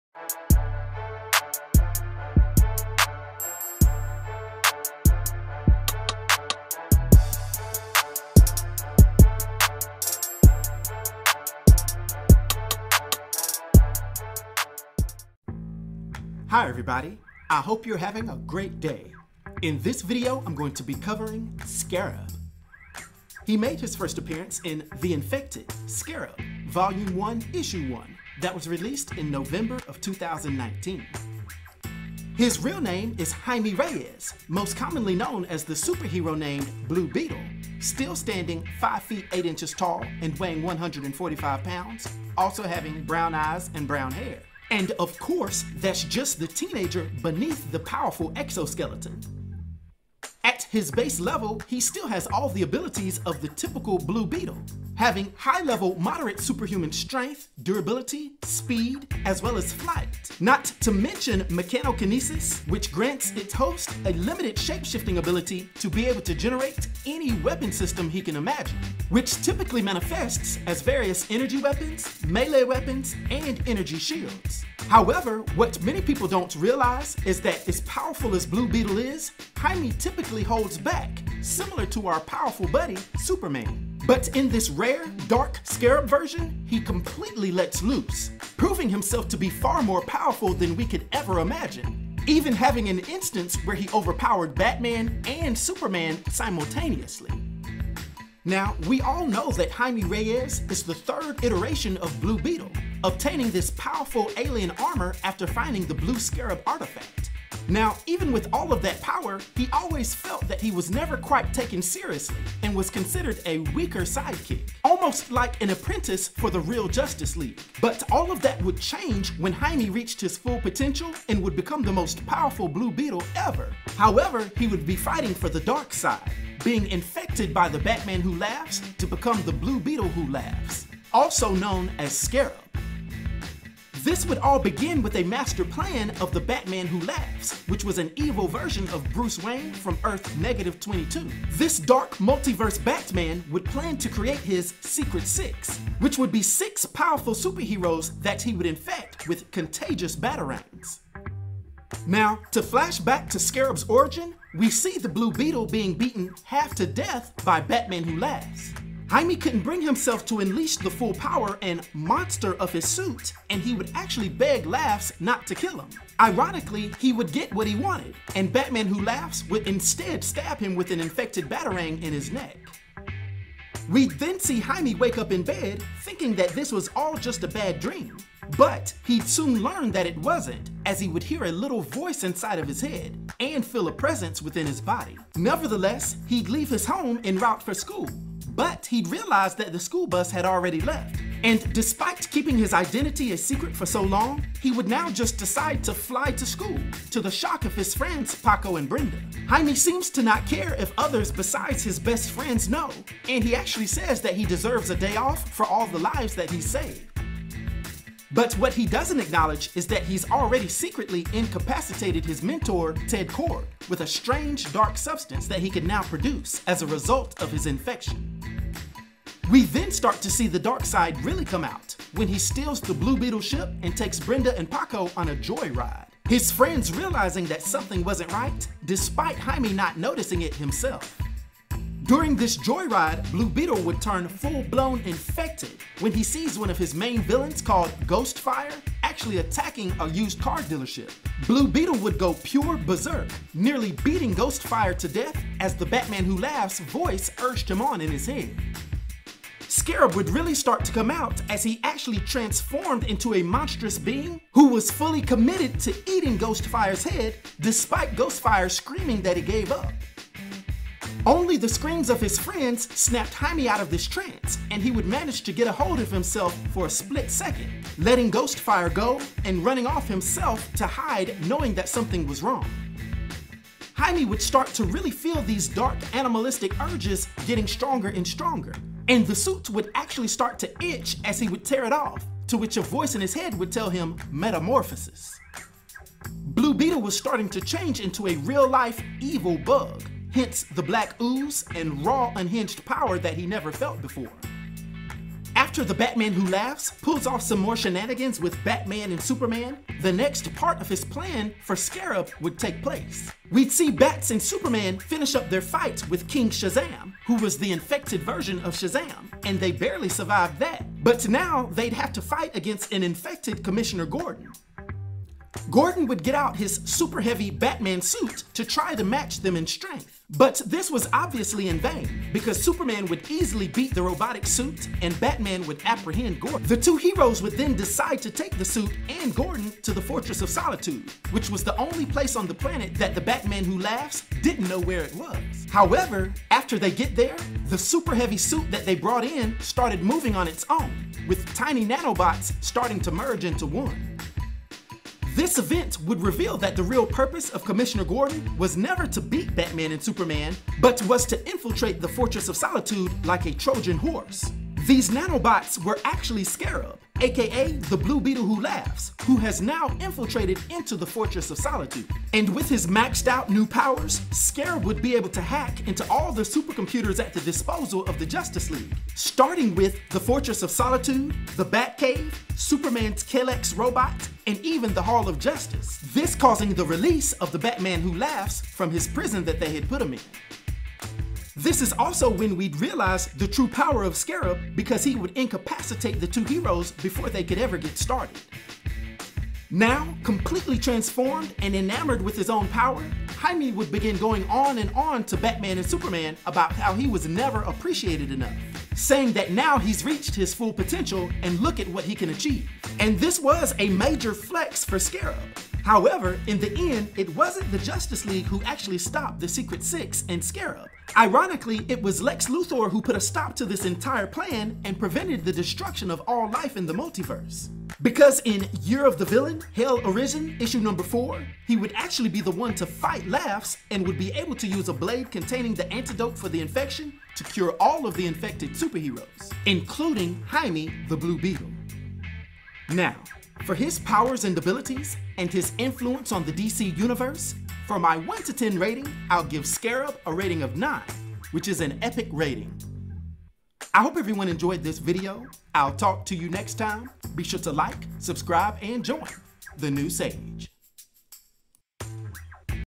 Who is DC Comics' Scarab? "Blue Beetle Who Laughs"
This video explores the abilities, powers, and origin of the villainous incarnation of the Blue Beetle DC Comics character named "Scarab" aka "The Blue Beetle Who Laughs". From Jaime Reyes being selected by The  Batman Who Laughs to be one of his minions known as the Secret Six aka The Infected, slowly beginning to transform into an extremely powerful yet evil bug form after being beaten and infected by specialized Batarangs by Laughs, realizing that the Dark Multiverse Batman had sway over him after losing control during a fight with his enemy named Ghostfire, being a key part in The Batman Who Laughs' plan to infiltrate Superman's Fortress of Solitude and the Batcave as well as the Hall of Justice, flexing his newfound unbridled power by temporarily subduing Superman and Batman at the same time, to being released from the control of Laughs by an unlikely ally in Lex Luthor; Let's take a look at this Dark Multiverse version of a beloved DC superhero, and the most powerful iteration of the magically-enhanced alien technology known as the Blue Beetle that has ever existed. This includes a short, canon character biography and my own opinionated "Sage Rating," which I use to give ratings of comic book and science fiction characters.